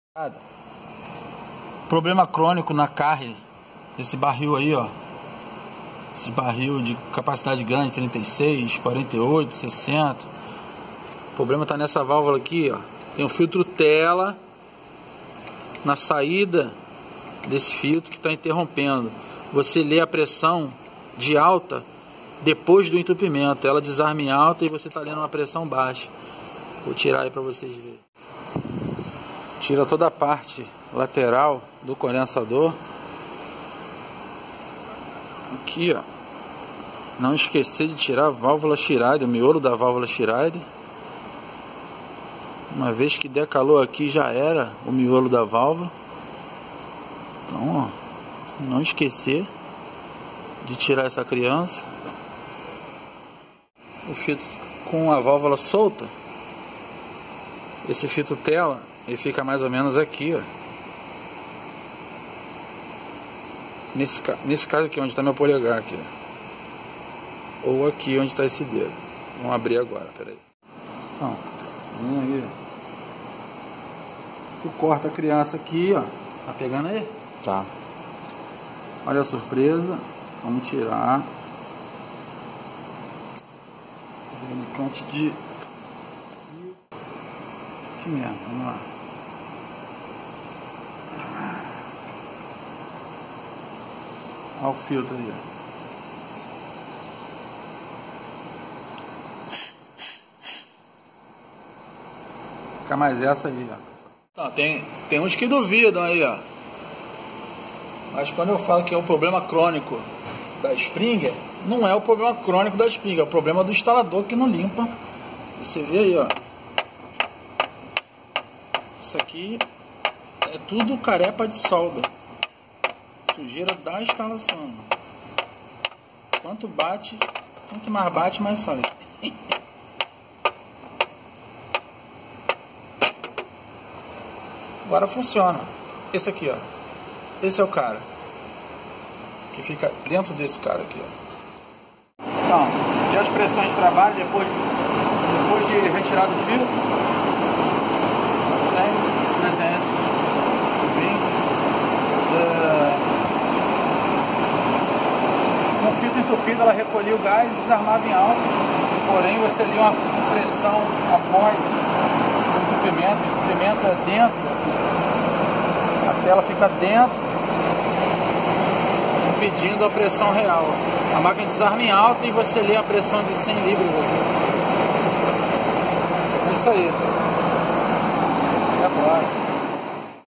Problema crônico de entupimento de filtro tela na válvula da linha de líquido 38 cc da carrier
Problema crônico causado pelo acumulo de sujeira e oxidação de solda sem fluxo de nitrogênio, muito comum em instalações onde não se atentam para as boas práticas na instalação.